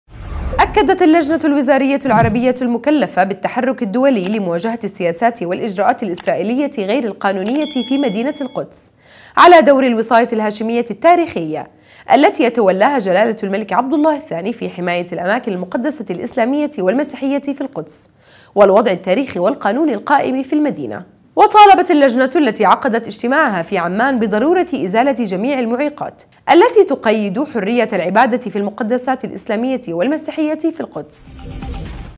الوزاريةُ العربيه تؤكدُ على دورِ الوصايةِ الهاشميةِ التاريخيةِ في القدس
أكدت اللجنةُ الوزاريةُ العربيةُ المكلفةُ بالتحركِ الدولي لمواجهةِ السياساتِ والإجراءاتِ الإسرائيليةِ غيرِ القانونيةِ في مدينةِ القدس / على دورِ الوصايةِ الهاشميةِ التاريخيه / التي يتولاها جلالةُ الملكِ عبدالله الثاني في حمايةِ الأماكنِ المقدسةِ الاسلاميةِ والمسيحيةِ في القدس / والوضعِ التاريخي والقانوني القائمِ في المدينه / وطالبت اللجنةُ التي عقدتْ اجتماعَها في عمان بضرورةِ إزالةِ جميعِ المعيقاتْ / التي تُقيدُ حريةَ العبادةِ في المقدسات الاسلاميةِ والمسيحيةِ في القدس